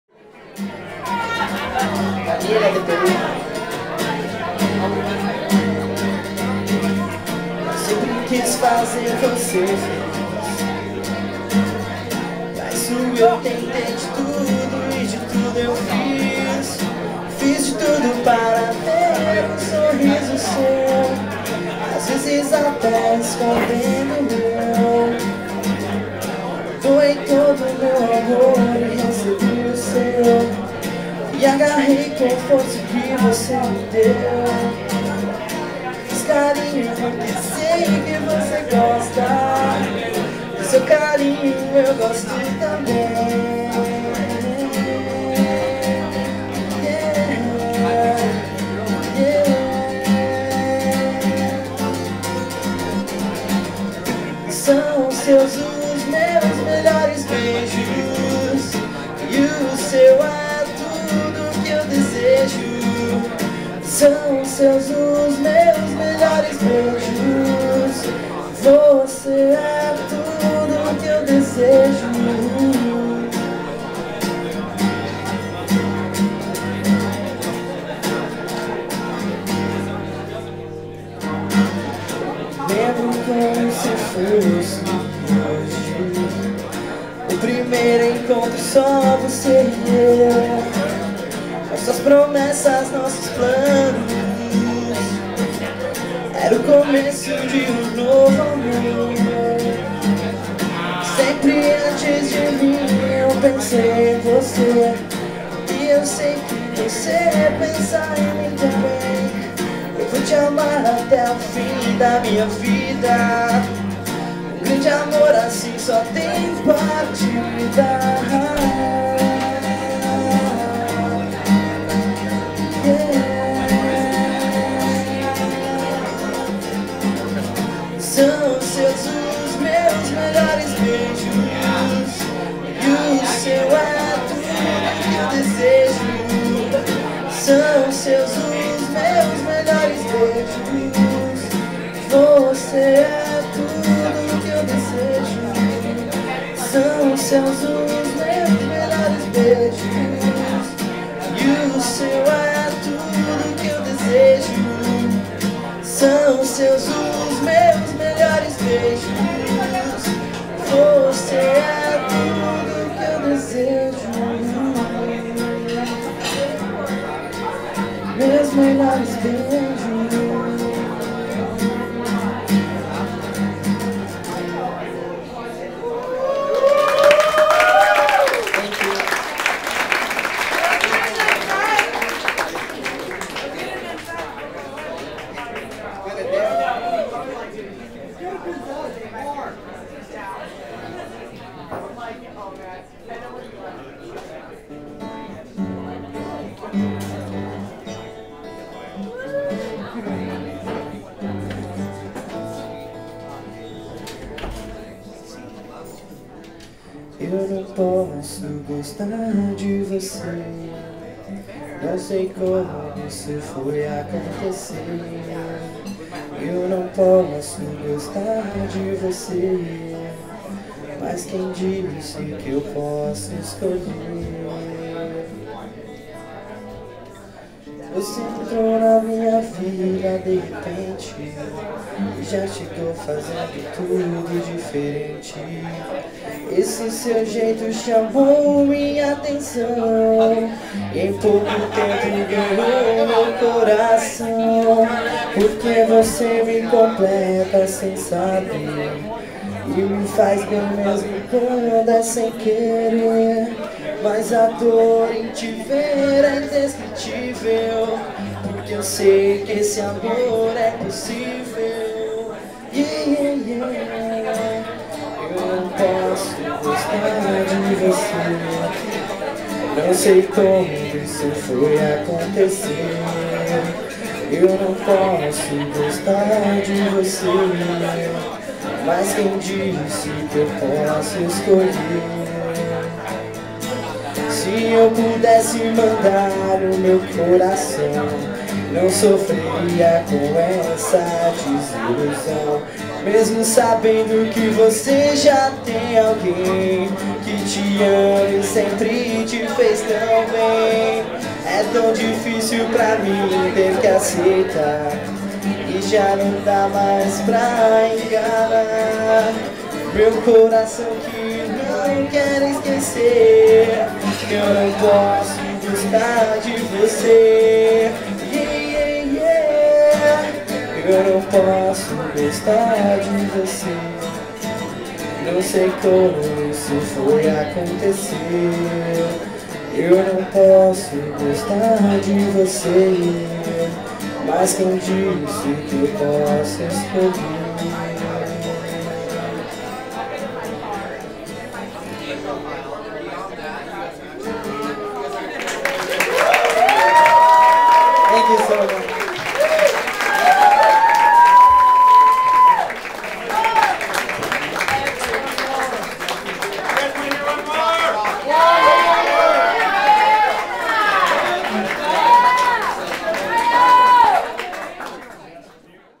Caio Zaplana @ The Sleeping Lady - Feb 4, 2009
Caio Zaplana performs at Jack Irving's Songwriter Showcase.